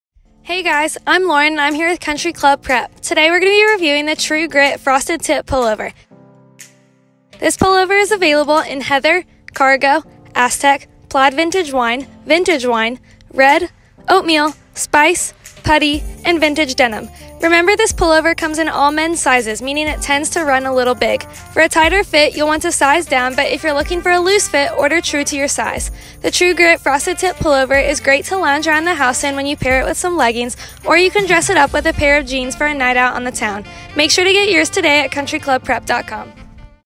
PRODUCT REVIEW: True Grit Pullovers
Country Club Prep interns Allison, Lindsey and Lauren review the True Grit Pullovers!

Shop similar styles and have your wildest prep dreams fulfilled